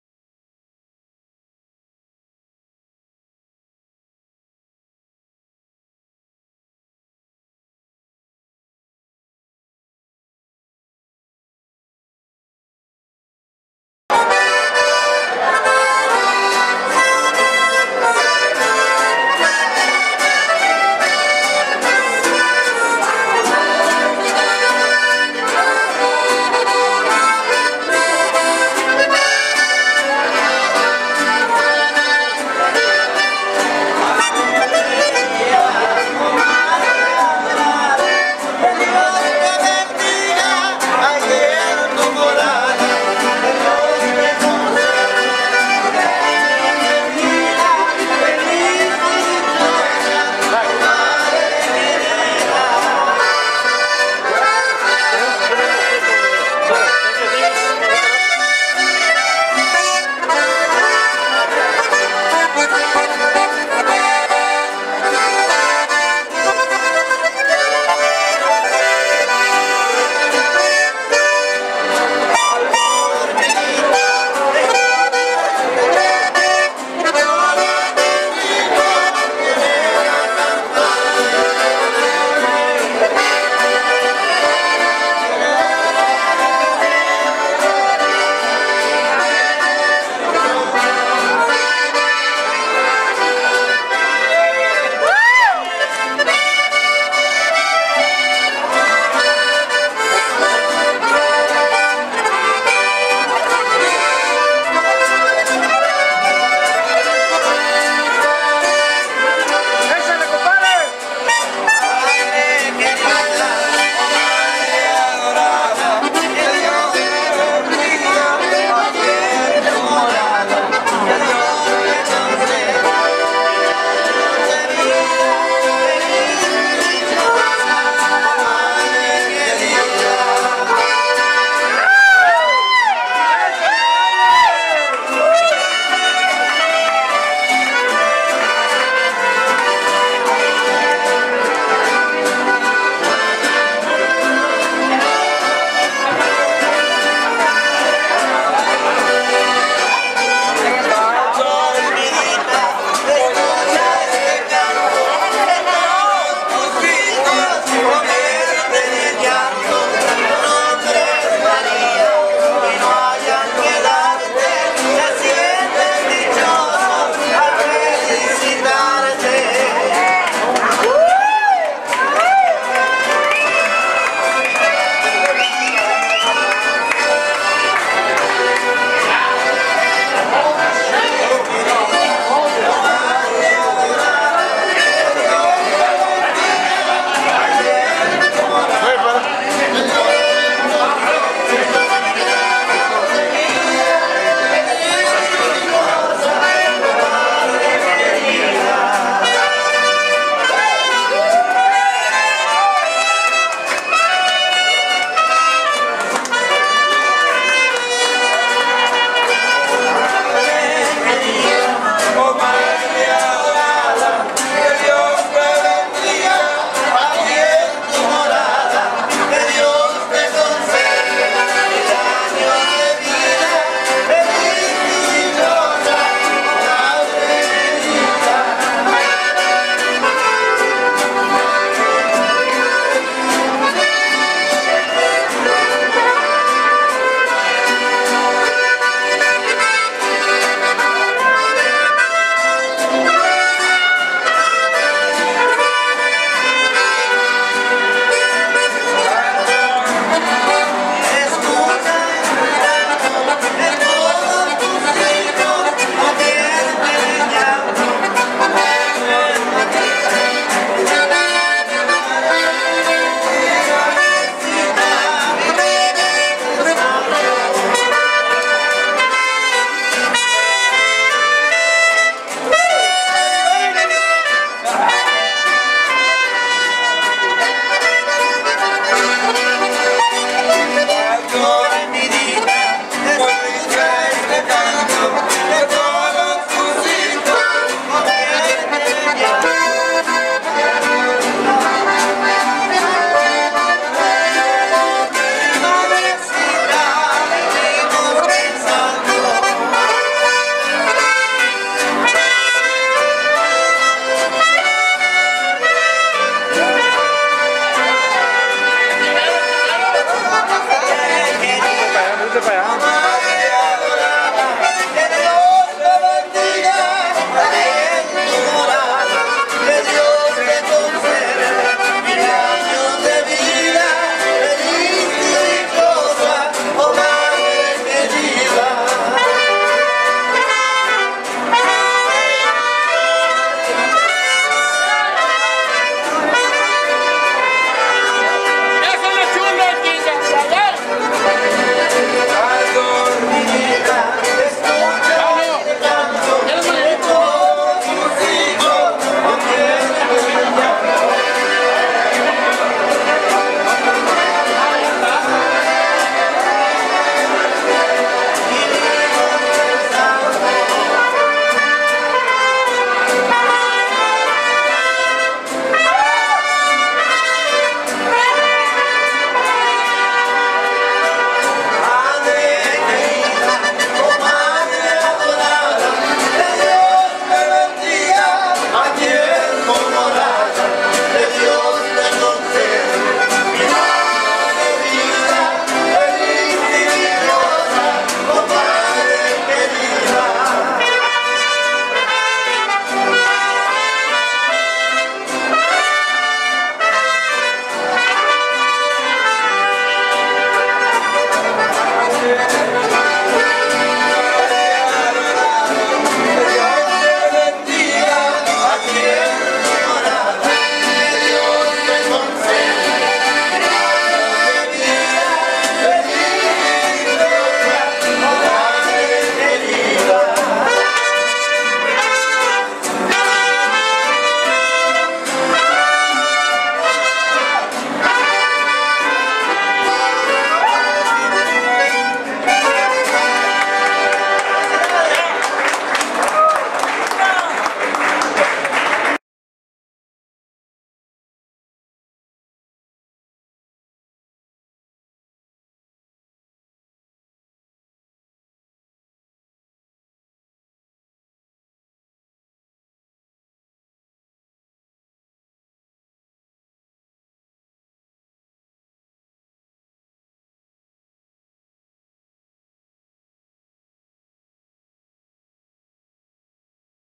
MOTHERS DAY PEPE MALDONADO Y RILEY Y LOS GILITOS
SINGING  TO ALL THE MOMS AT LA LOMITA PARK 05/08/2016